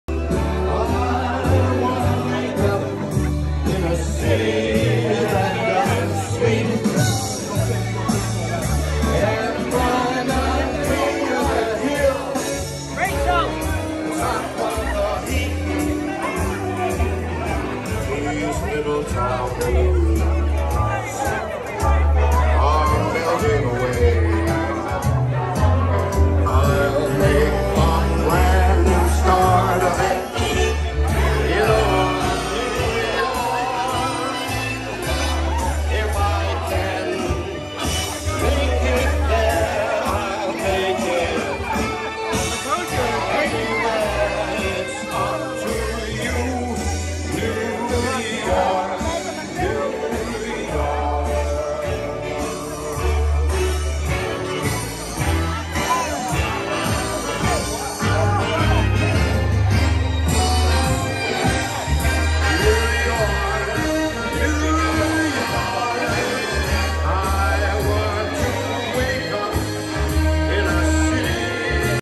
Wedding - Sinatra Finish
Getting all the guests together for the finale!